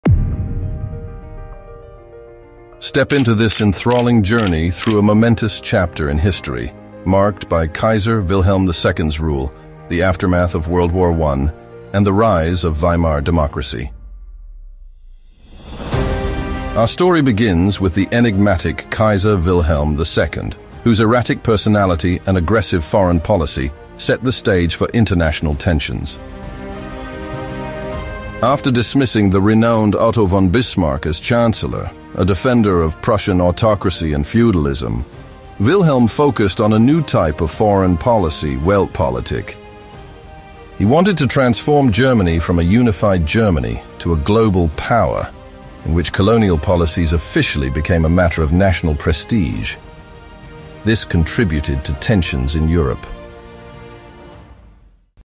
Kaiser Wilhelm II: Introduction. Part 1/6
A critical era, spanning from Kaiser Wilhelm II’s reign to the Weimar Republic's formation in the wake of World War I. Explore the intense geopolitics, economic turmoil, alliances and shifting political landscapes that defined this period. Witness how these historical events still influence our contemporary global landscape.